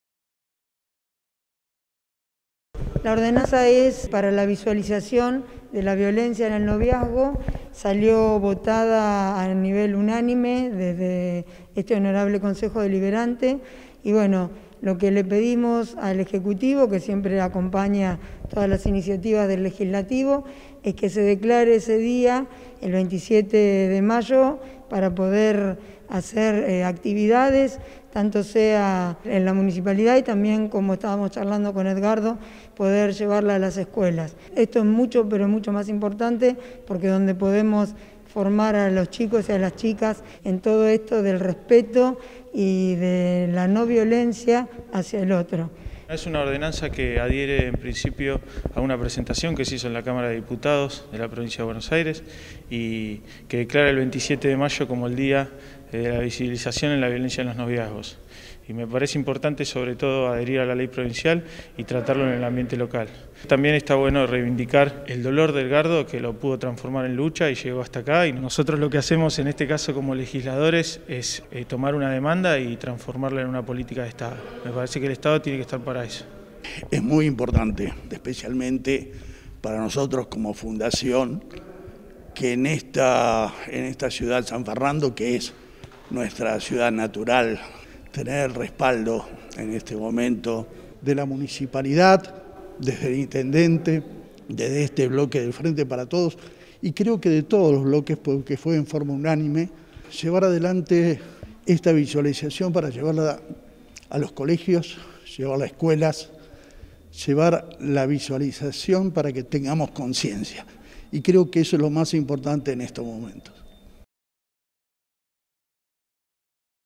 El HCD declaró el 27 de mayo Día de la Visibilización y Lucha contra la Violencia en los Noviazgos
El proyecto de ordenanza aprobado por unanimidad adhiere a la campaña de difusión sobre las implicancias de los noviazgos violentos y habilita al Municipio a desarrollar acciones de concientización en espacios públicos y escuelas de San Fernando. La fecha recuerda el femicidio de Carolina Aló, sanfernandina asesinada por su pareja en 1996, de 113 apuñaladas.